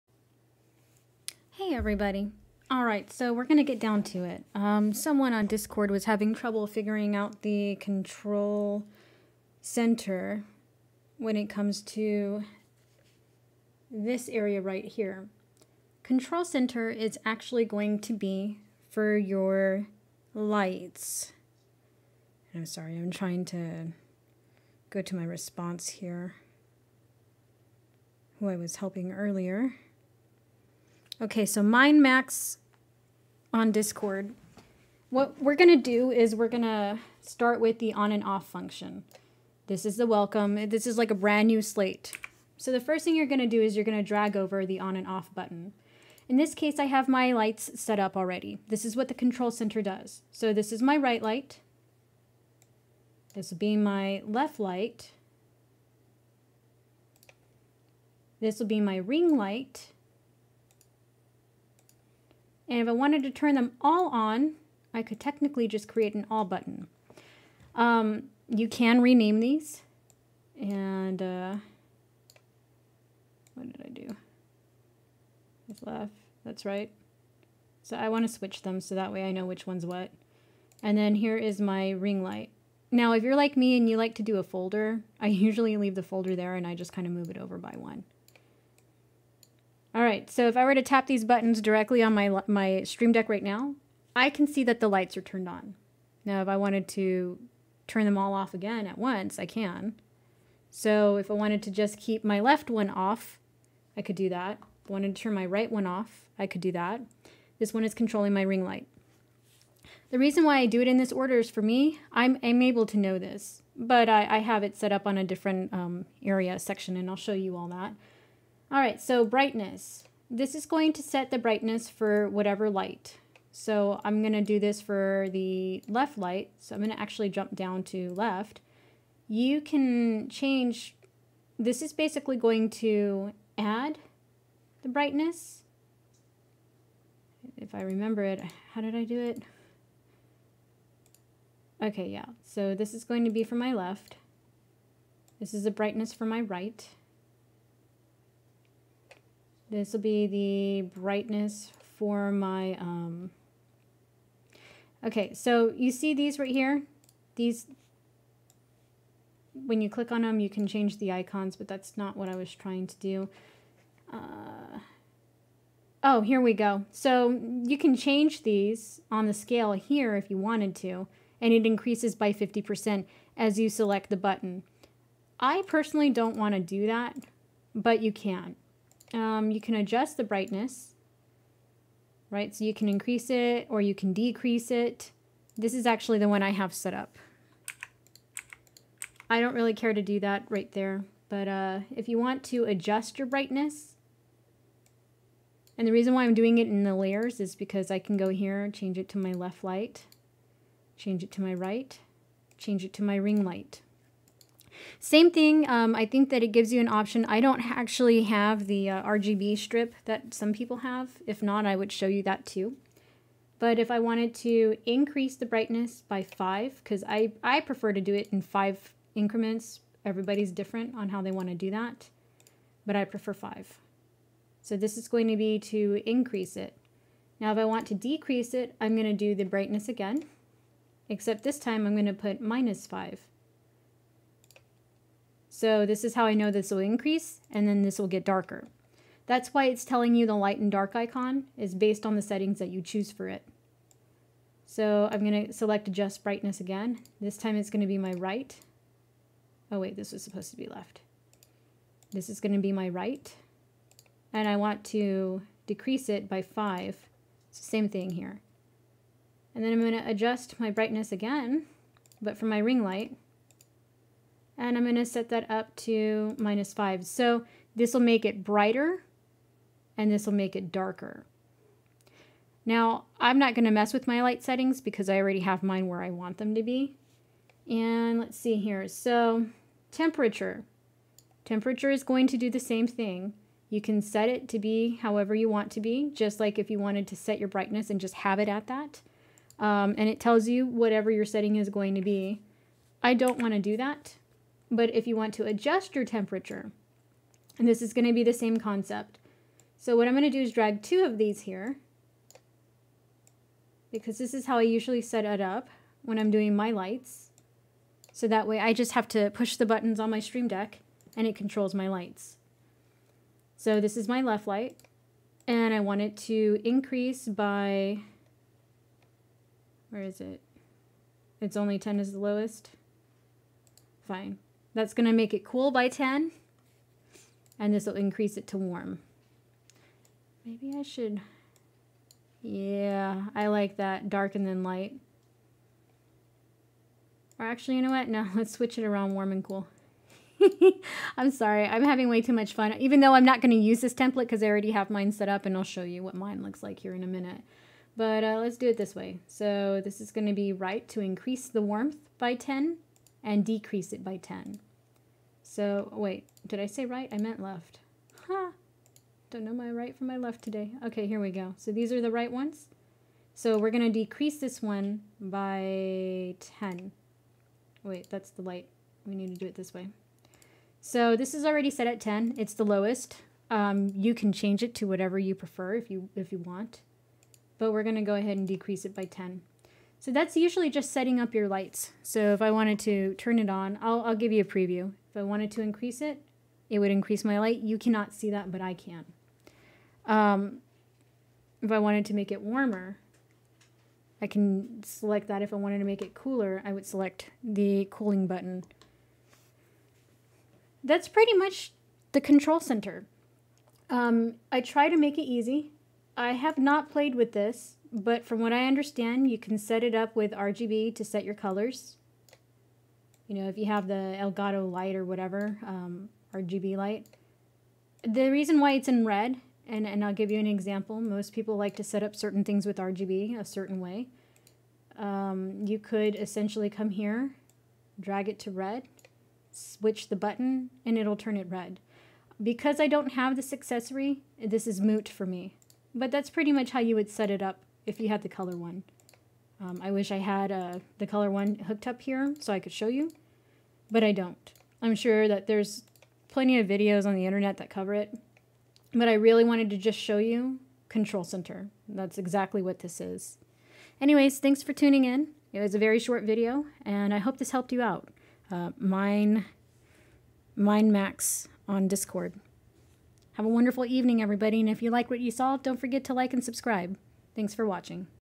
Using Control Center on your Elgato Streamdeck
I did this setup really quick on my Streamdeck to show you how simple it is to set up your elgato air/key or ring lights. This setup can be done for both Mac & Windows.This is my exact setup, and very quick and simple to do. A drag and drop feature to the streamdeck mapping, and what functions I want it to do will be displayed on the video. Disclaimer: I do not have the RGB strip, but you can set up custom colors per input, however, I am sure there is more to be done with the RGB strip, but I can't really provide details on that as I don't own one.